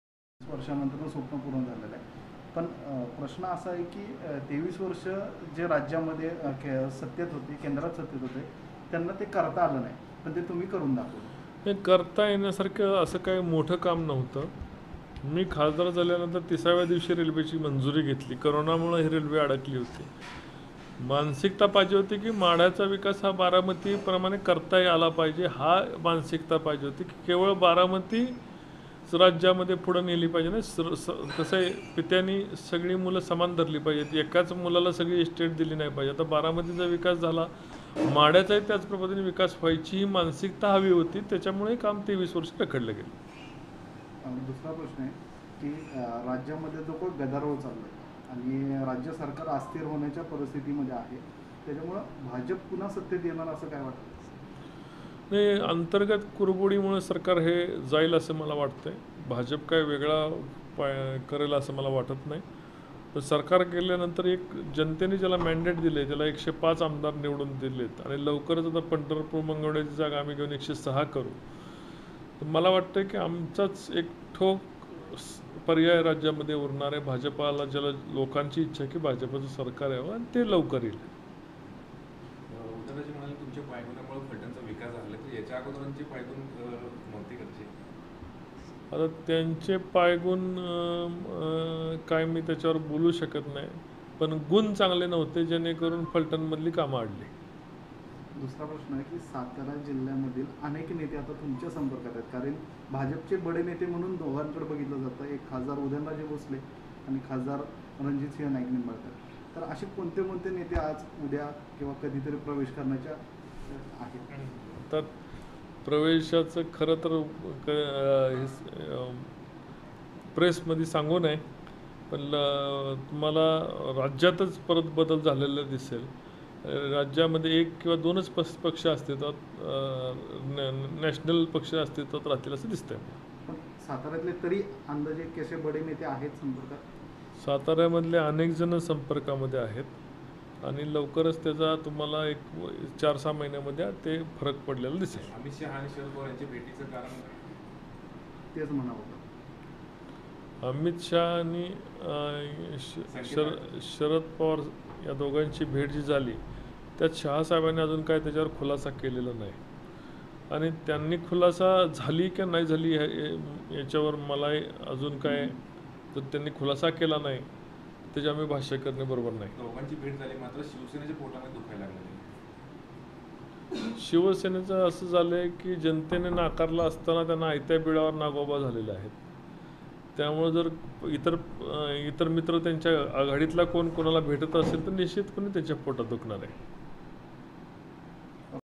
भाजपचे गल्लीसह दिल्लीत ही ऑपरेशन लोटस : सातारचे अनेक नेते भाजपच्या संपर्कात- रणजितसिंह नाईक निंबाळकर
सातारा- राज्यामध्ये महाविकास आघाडीचे अनेक कारनामे बाहेर येत असताना भाजप मात्र गल्लीसह दिल्लीमध्ये महाराष्ट्राच्या राजकारणावर ऑपरेशन लोटस राबवताना दिसत आहे.. त्याचाच एक भाग म्हणून सातारा जिल्ह्यातील अनेक नेते भाजपच्या संपर्कात असल्याचा गौप्यस्फोट माढा मतदारसंघाचे खासदार रणजितसिंह नाईक-निंबाळकर यांनी केल्याने येणाऱ्या काळात राजकीय वर्तुळात खळबळ उडवणारे विधान केले आहे..

राज्यातील महाविकास आघाडीचे सरकार भाजप पाडत नसून ते त्यांच्या कर्तृत्वानेच पायउतार होणार आहेत.. त्यामुळे पंढरपूरच्या पोटनिवडणुकीच्या जागेवर भाजपच विजय होईल आणि भाजप 155 जागेसह पुन्हा एकदा राज्यात सत्तेत येईल असे विधान रणजितसिंह नाईक-निंबाळकर यांनी केले..

राज्याच्या राजकारणामध्ये शिवसेनेला जनतेने डावलले असताना देखील इतर पक्षाच्या साहाय्याने शिवसेना आयत्या बिळात नागोबा सारखी सत्तेत बसली असून महाविकास आघाडीतील इतर घटक पक्ष भाजपच्या वरिष्ठ नेत्यांशी गाठीभेटी घेत असल्याने शिवसेनेच्या पोटात दुखू लागले असल्याचा टोला खासदार रणजितसिंह नाईक निंबाळकर यांनी केला आहे